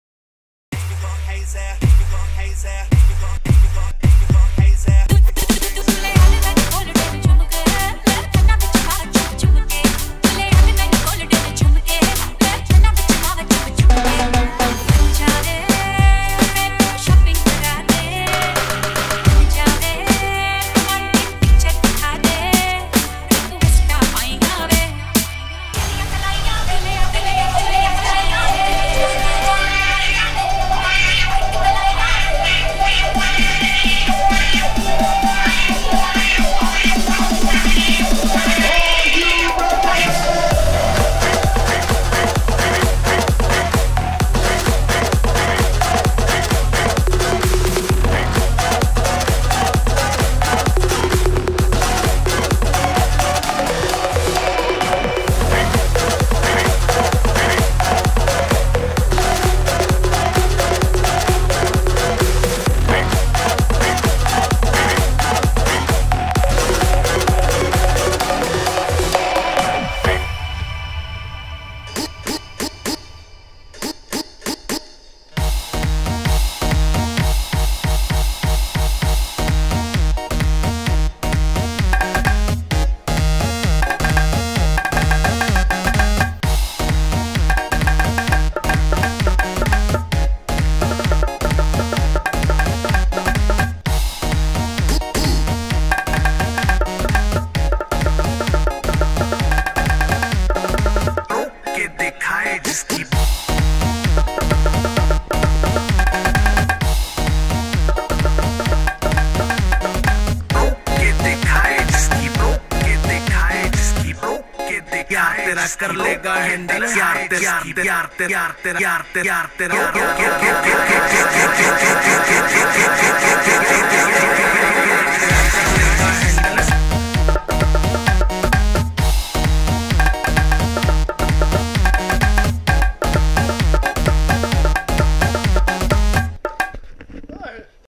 MIDI JACKET Superkid 4.1. WEREABLE Music Instrument
Bulbul the Sensorman playing with Superkid Midi jacket 4.1 and  BOLLYWOOD MUSIC!